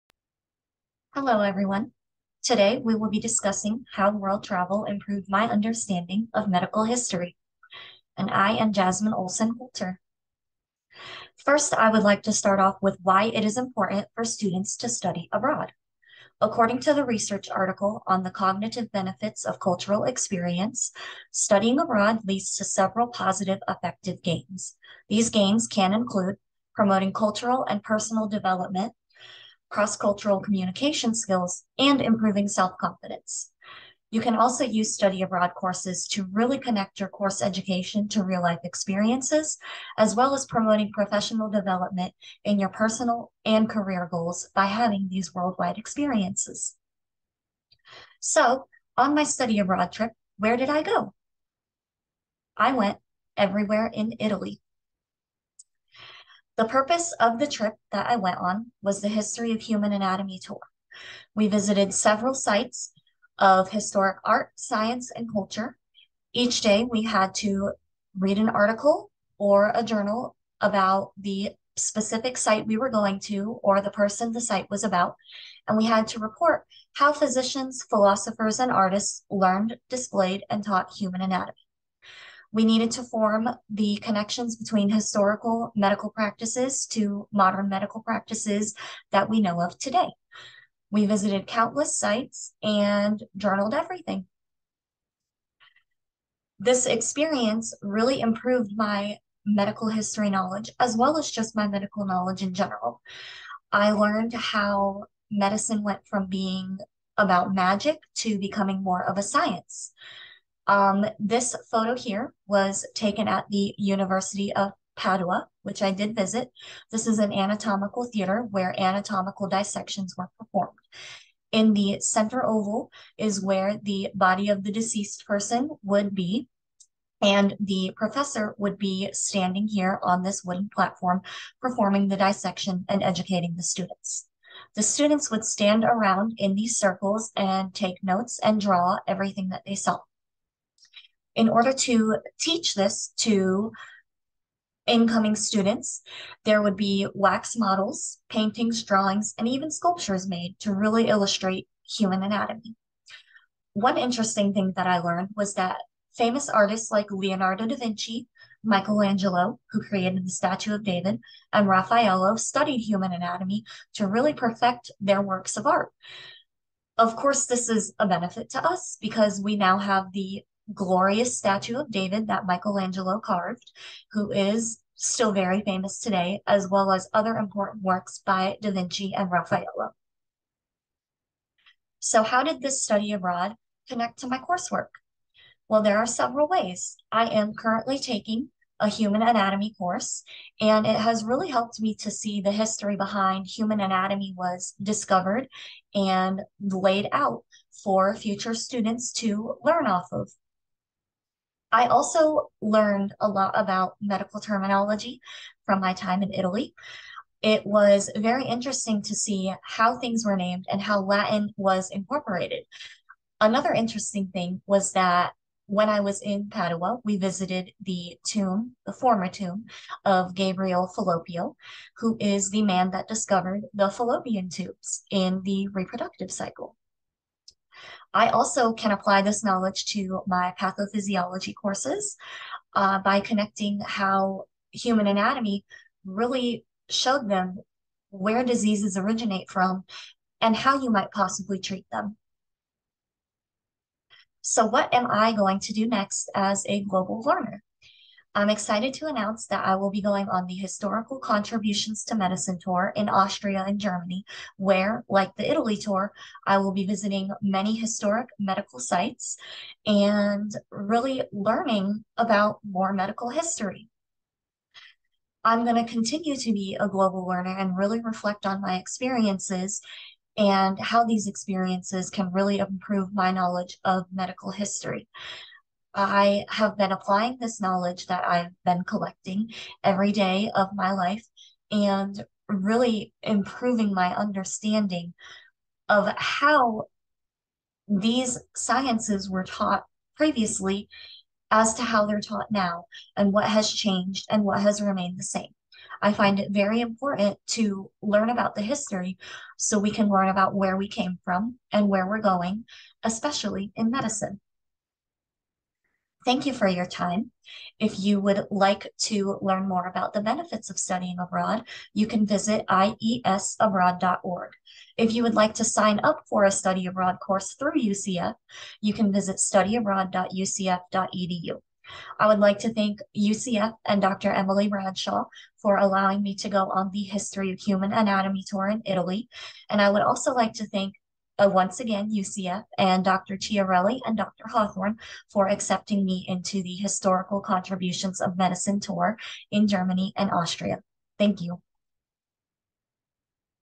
How World Travel
UCF High Impact Course Presentation (Global Learning)
How did my trip to Italy impact my learning and how can future study abroad trips help me.